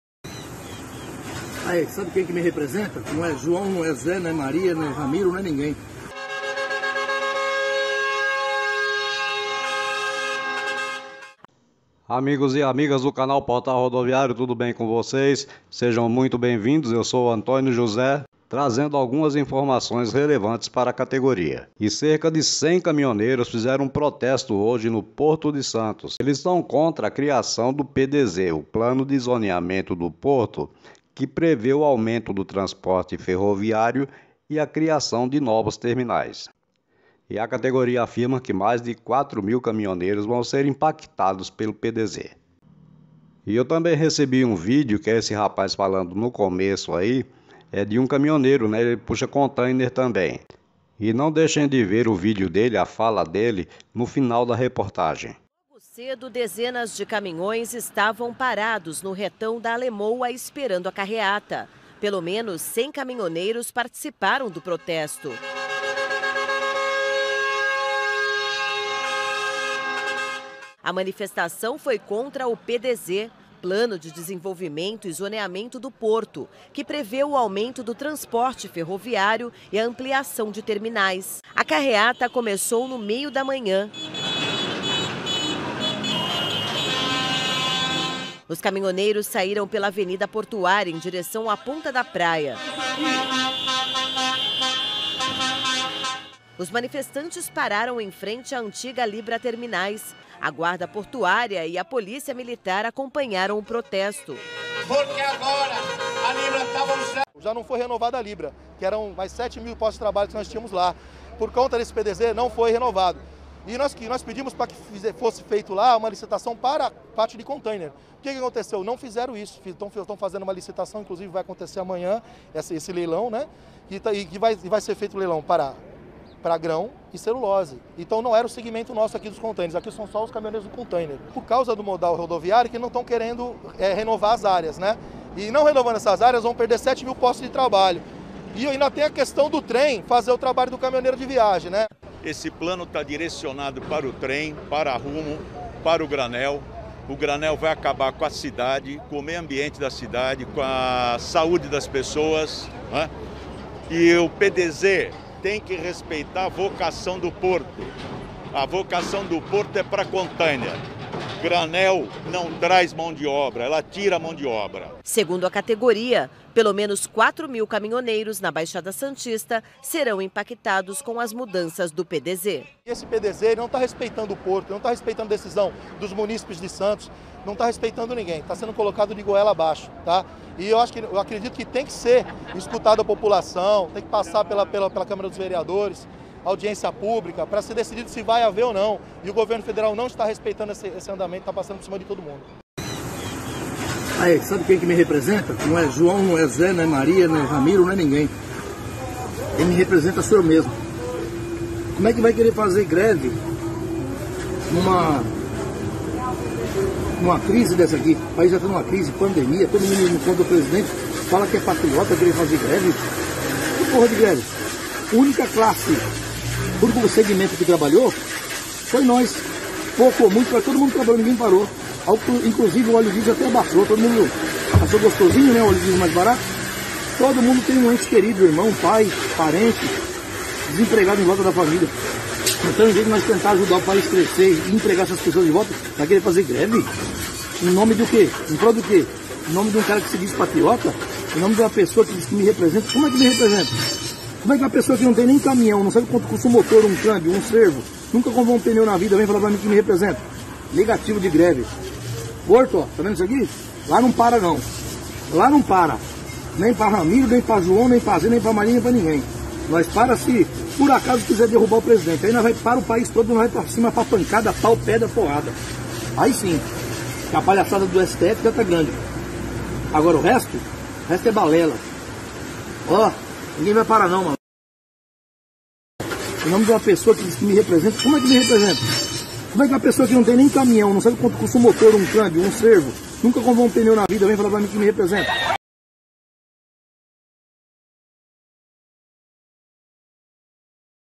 Caminhoneiros autônomos protestam no Porto de Santos
Caminhoneiros autônomos do Porto de Santos fizeram uma carretada na manhã desta quinta-feira (27) pela zona portuária. A categoria protestou contra o Plano de Desenvolvimento e Zoneamento (PDZ) do cais santista e contra o arrendamento da área anteriormente ocupada pelo Grupo Libra, na Ponta da Praia,
Nosso Email para assuntos comerciais e parcerias